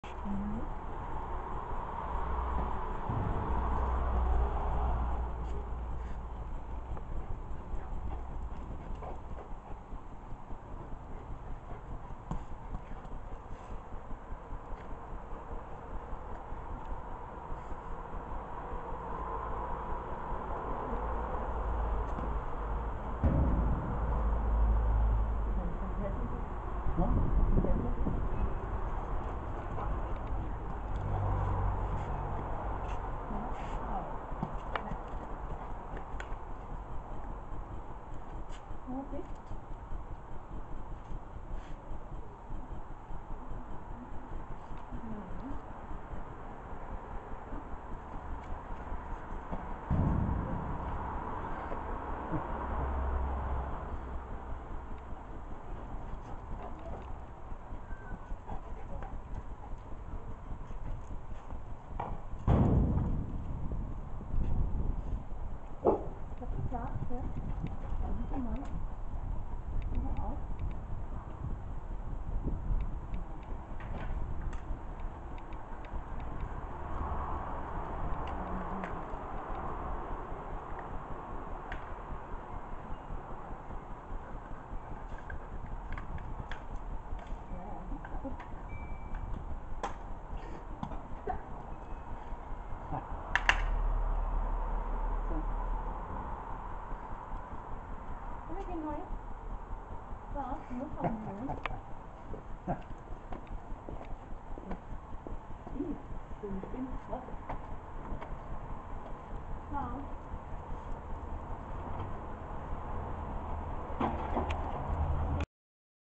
hunde spielen
Hunde spielen mit Tennisball - meine Nichte wirft den Ball